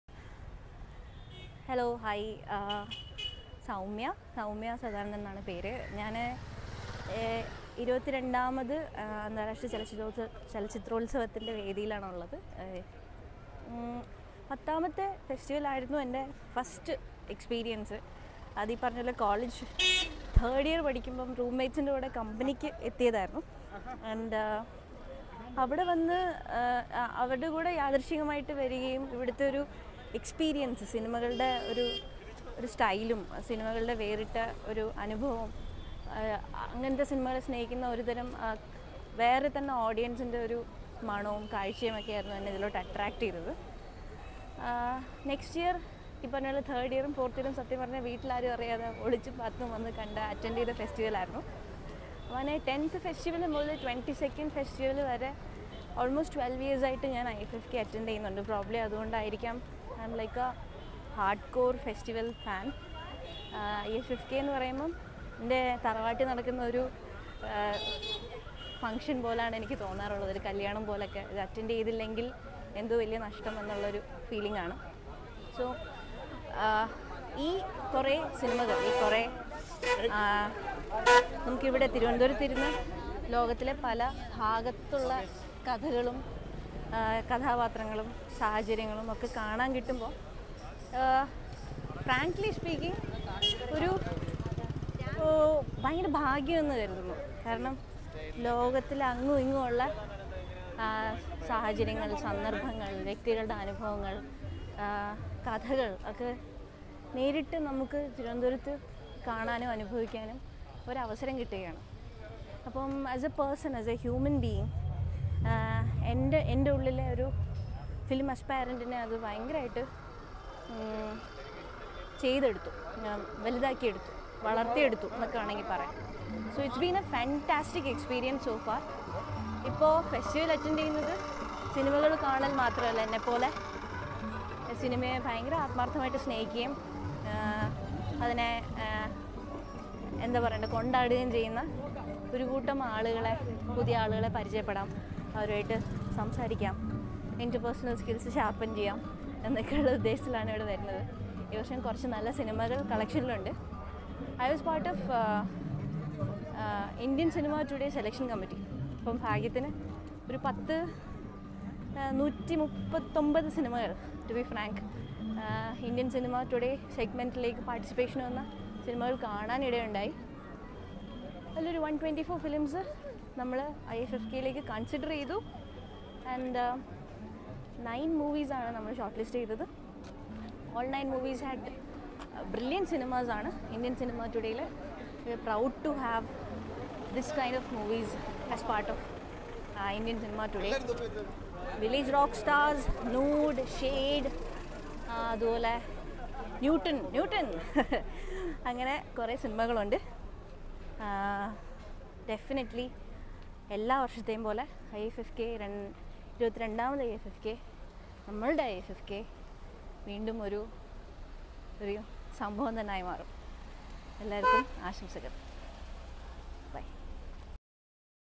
Soumya Sadanandan (Documentary director )in IFFK| IFFk 2017
ചലച്ചിത്രമേള തന്നെ രൂപപ്പെടുത്തിയതെങ്ങനെ? ദേശീയ പുരസ്‌കാരം നേടിയ ഡോക്യുമെന്ററി സംവിധായിക സൗമ്യ സദാനന്ദന്‍ പറയുന്നു